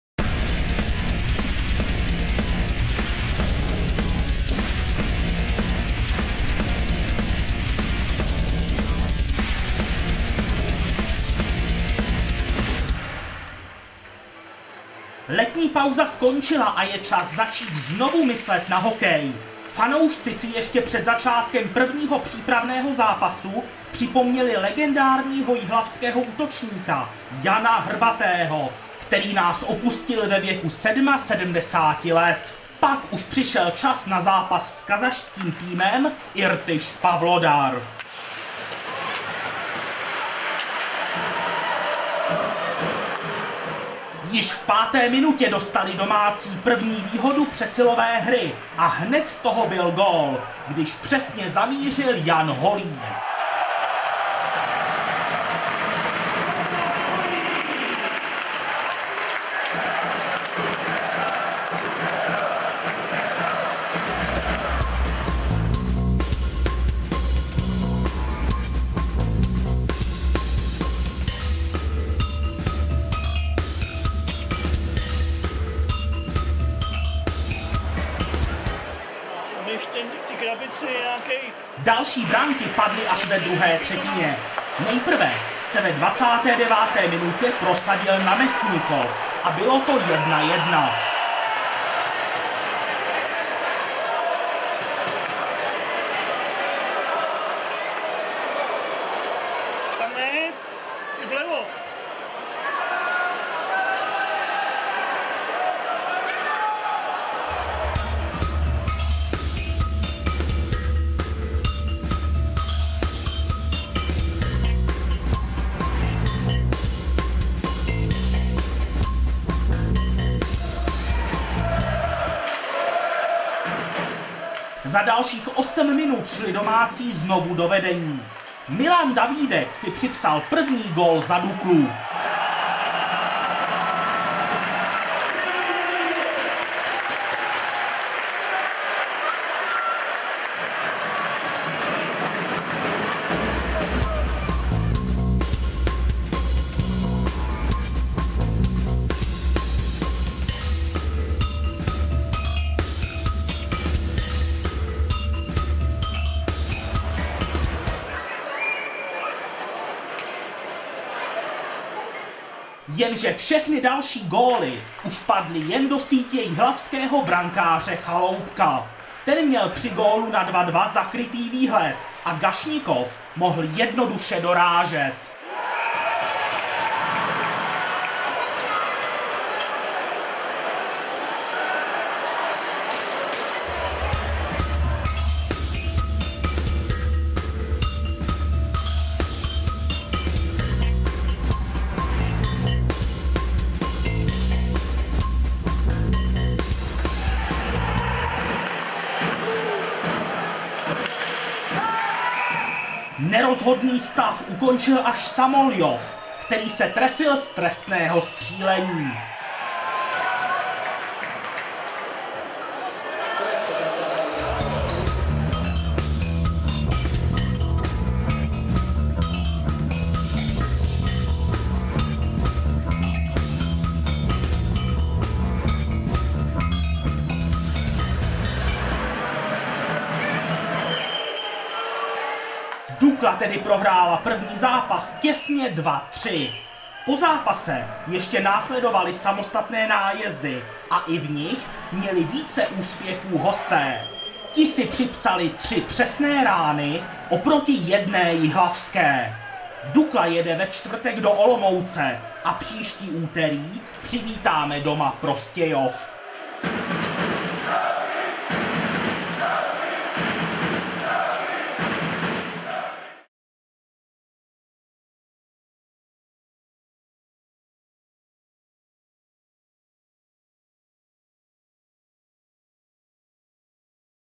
1. přípravný zápas (6.8.2019) HC Dukla Jihlava - HC Irtyš Pavlodar (KAZ)
Sestřih z prvního přípravného zápasu.